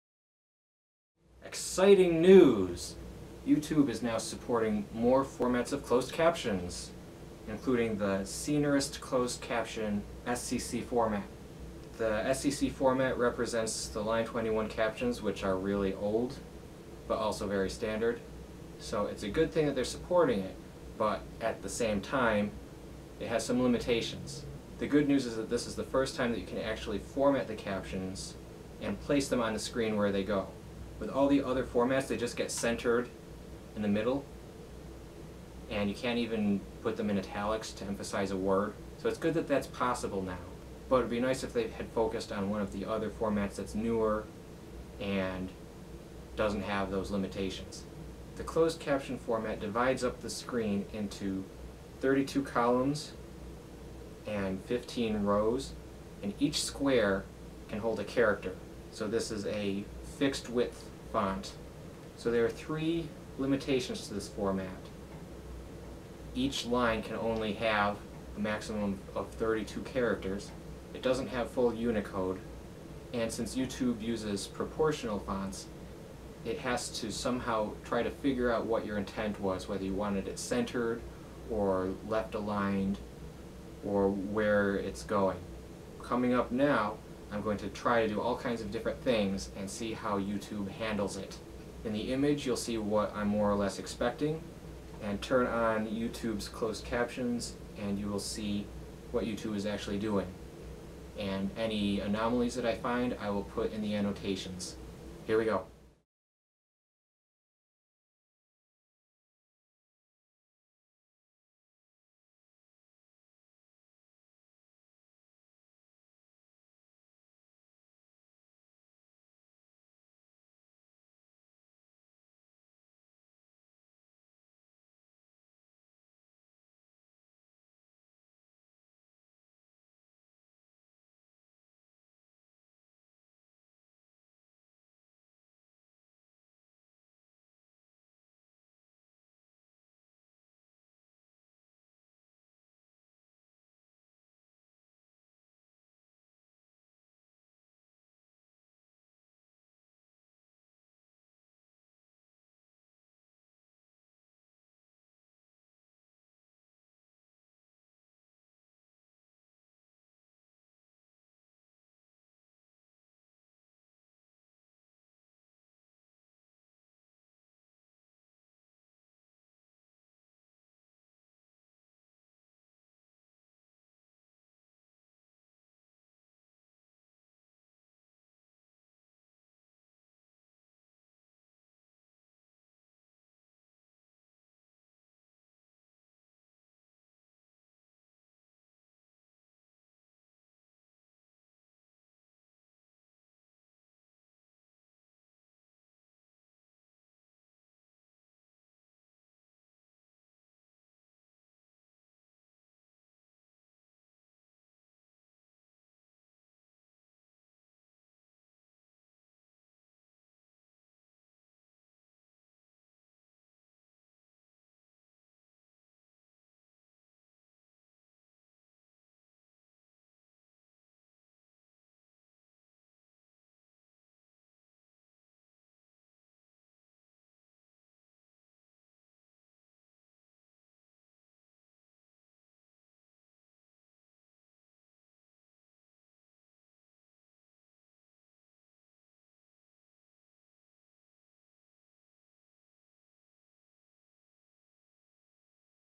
YouTube supports Scenarist Closed Captions... but how well?
YouTube now supports SCC caption files, and several others.  This format opens up new possibilities, but how well is it implemented?  How does YouTube work with the limitations?